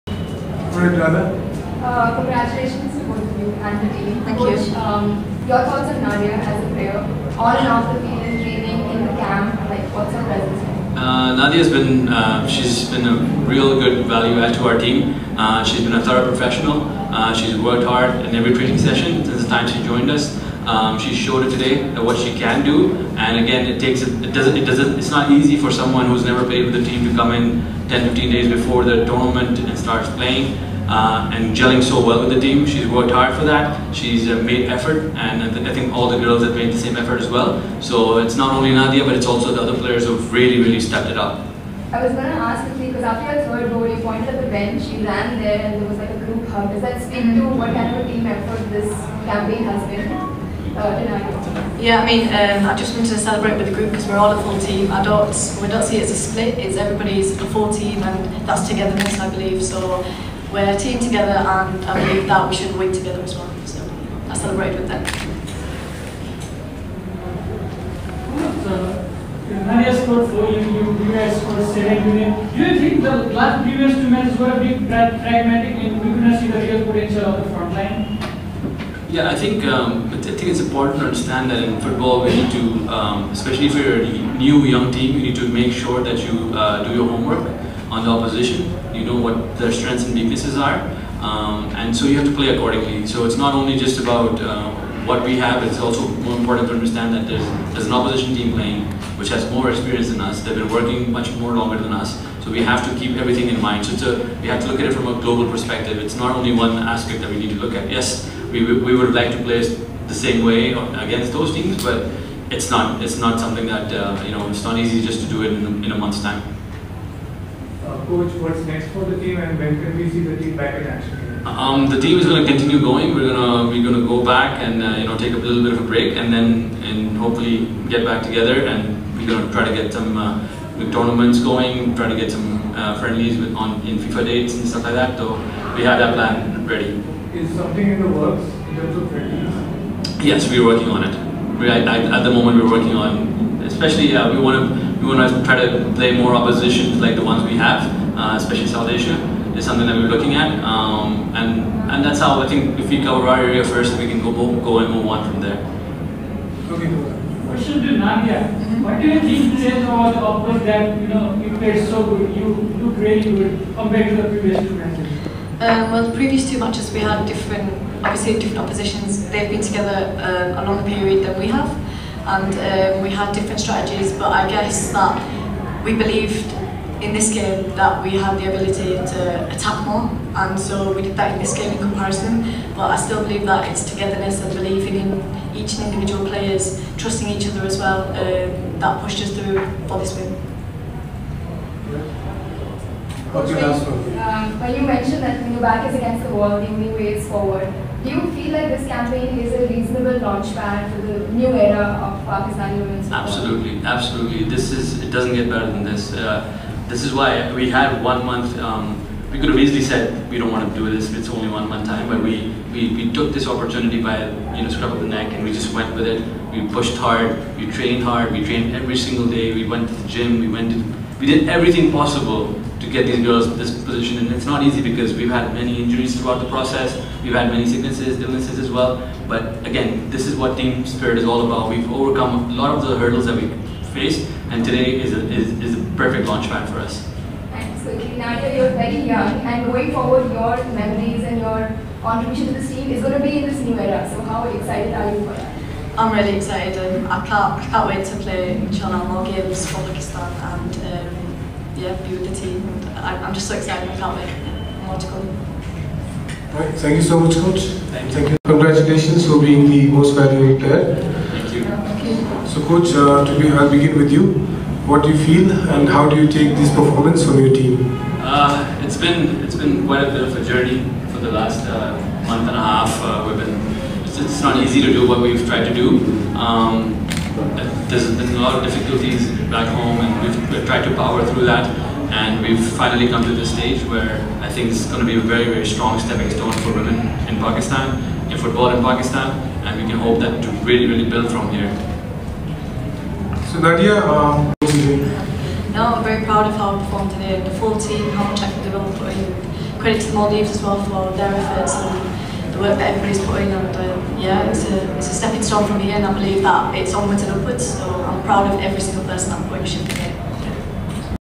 Post Match Press Conference | Adeel Rizki - Nadia Khan | MDV vs PAK | SAFF Championship 2022
Post Match Press Conference | Adeel Rizki - Nadia Khan | MDV vs PAK | SAFF Women's Championship 2022
🇲🇻Maldive 0 : 7 🇵🇰Pakistan